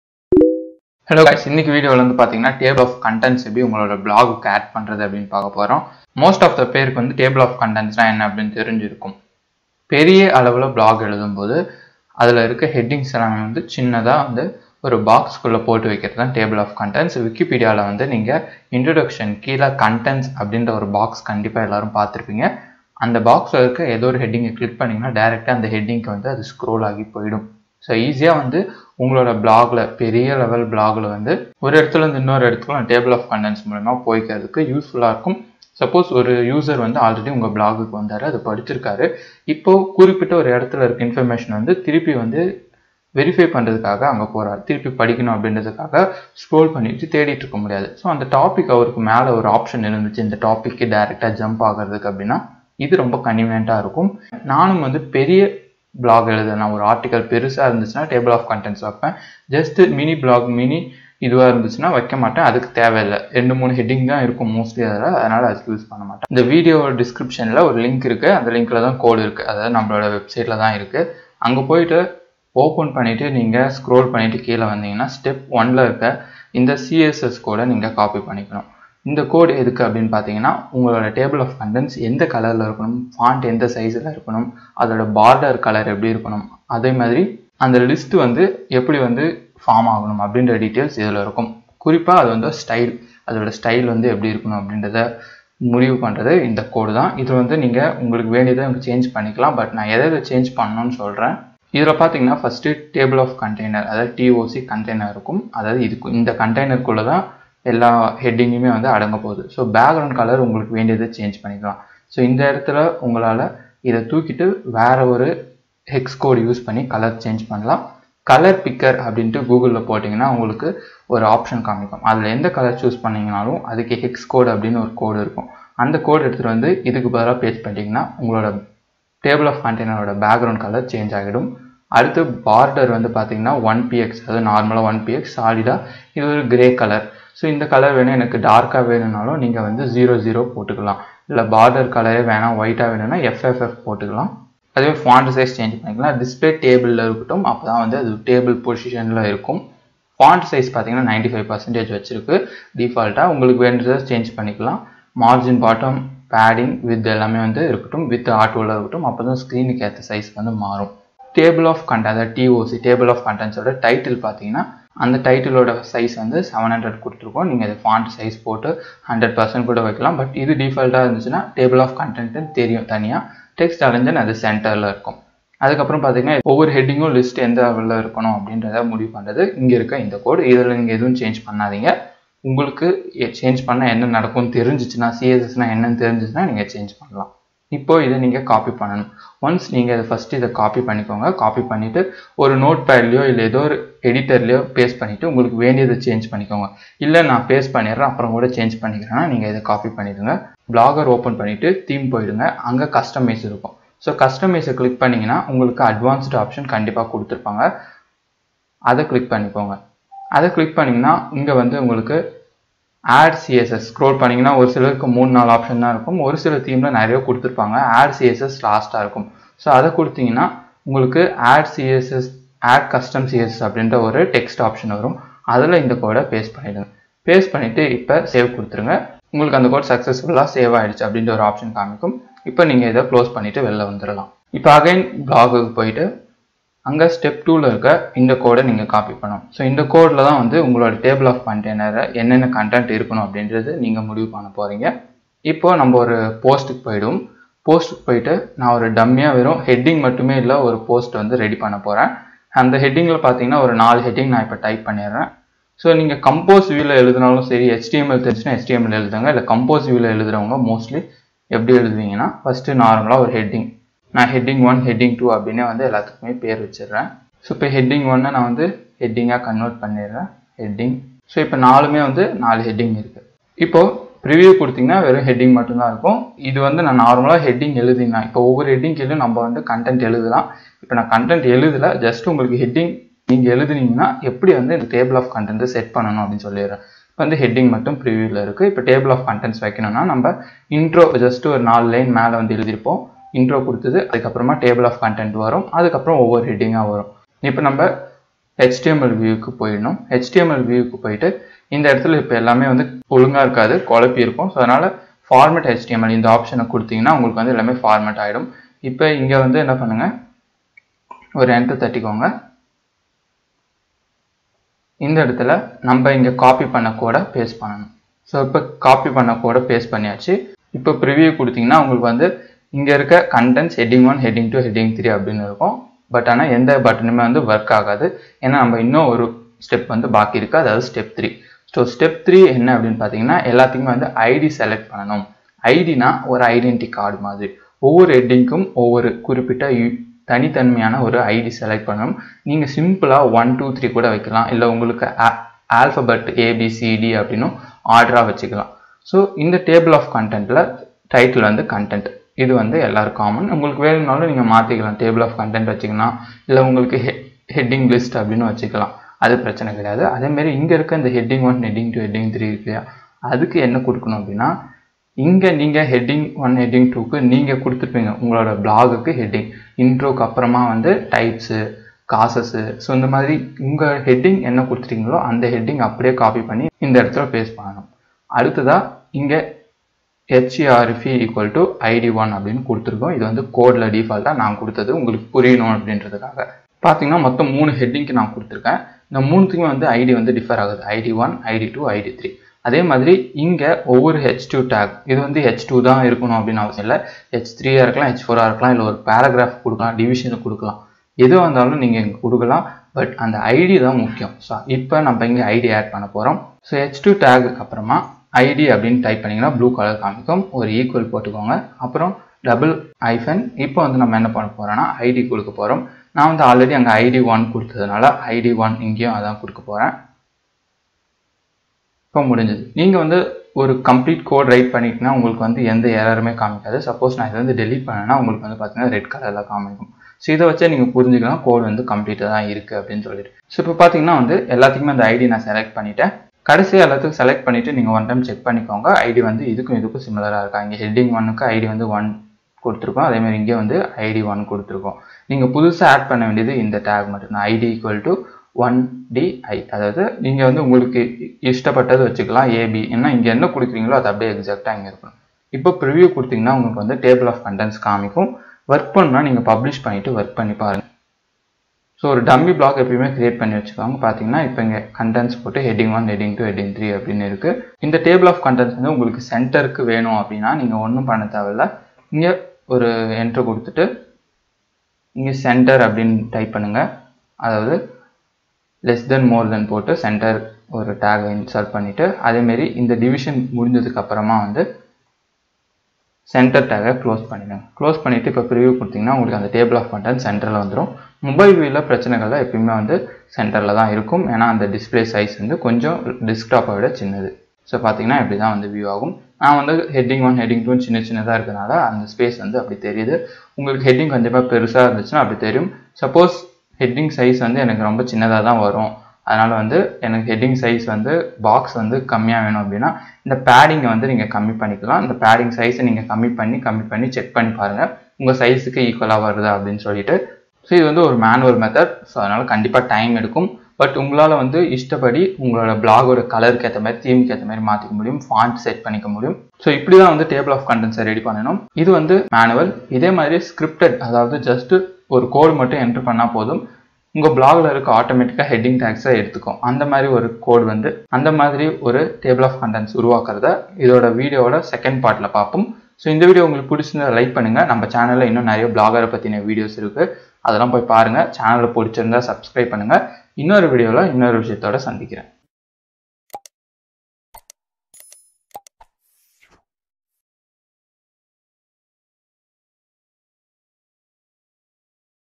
How to add "Table of Content" in blogger (Manual Method) | Blogger Series
Source : 2021/02/table-of-contents-script-blog-tips-in.html

Time Stamp
0:00 Intro - What is TOC?
1:06 Step 1 - copy CSS code
3:56 Step 2 - Table code
5:30 Step 3 - Set ID Value
8:50 Conclusion

🌐 Visit us :

Videosஐ தவறவிடாமல் பார்க்க, 🔔 பட்டனை ON செய்யவும்.